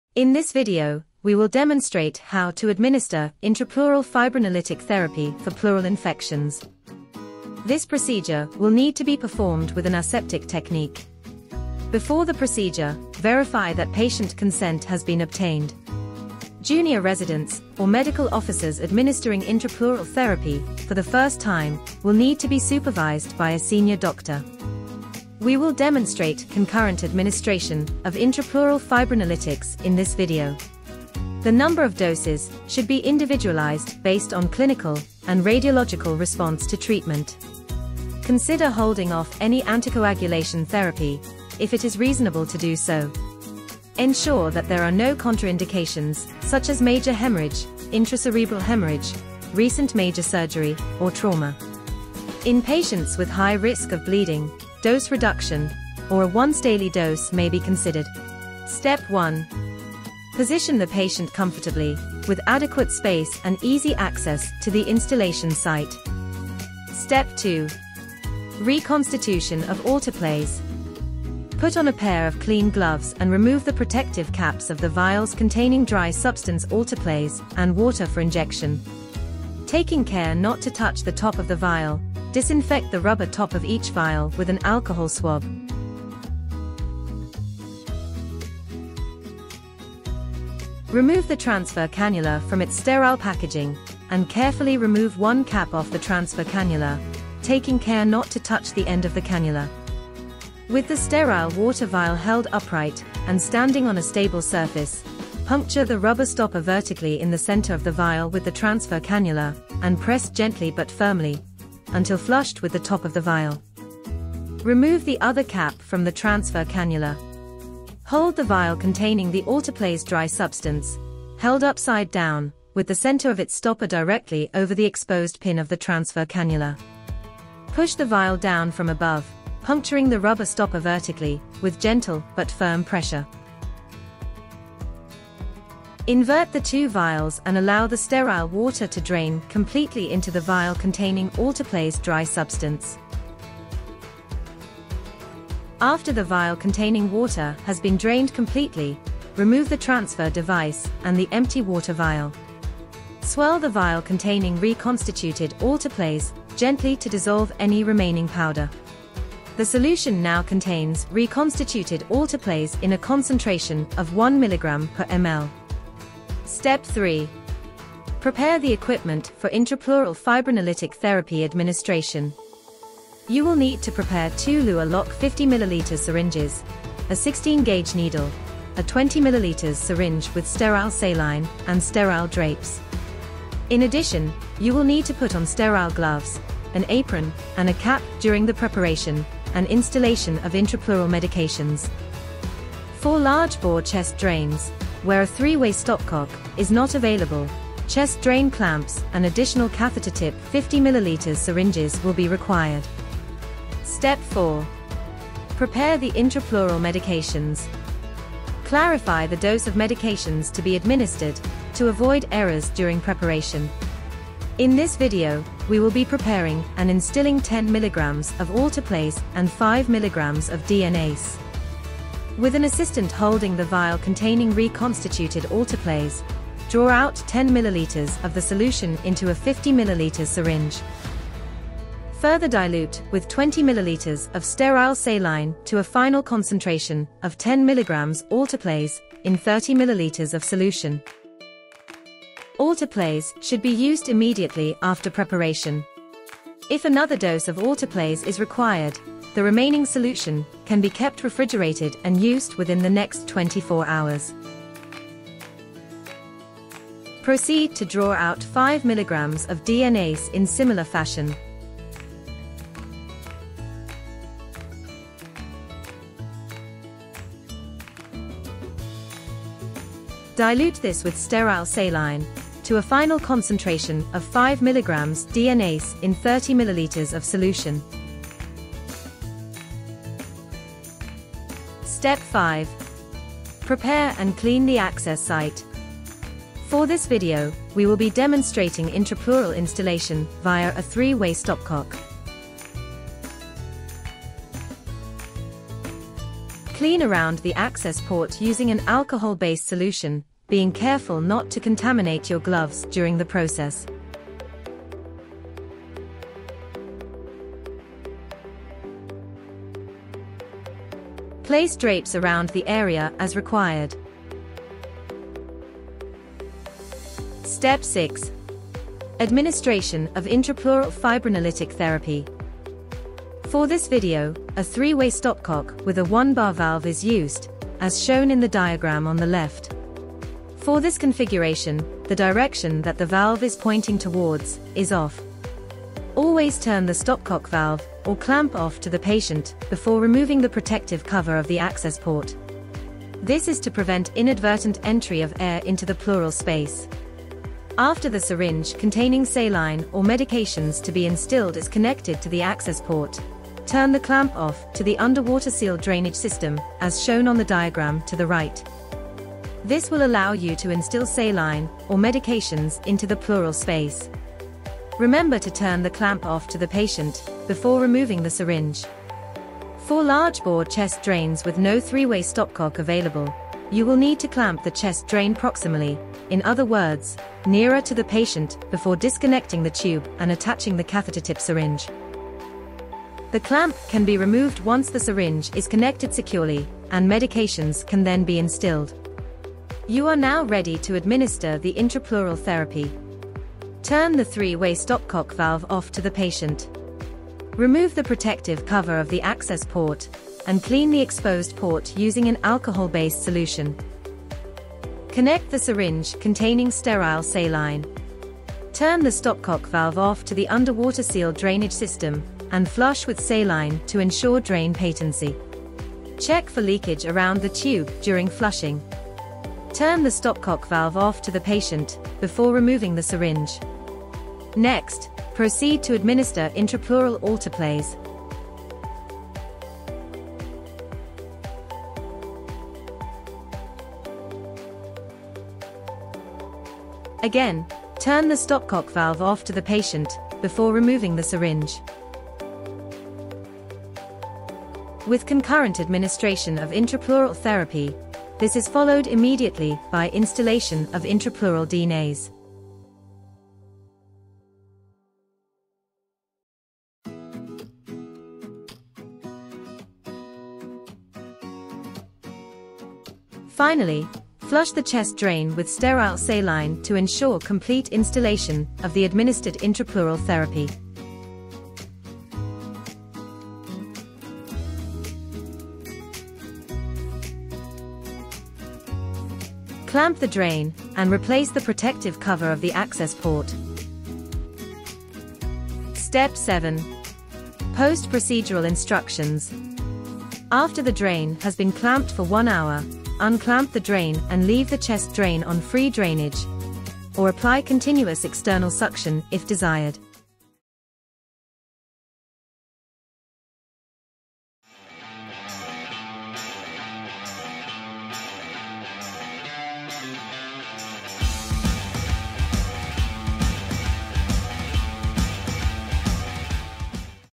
Intrapleural therapy for pleural infection
Intrapleural therapy is an important treatment option for pleural infections. This video demonstrates how to prepare and instill intrapleural fibrinolytics (alteplase) and deoxyribonuclease (DNAse)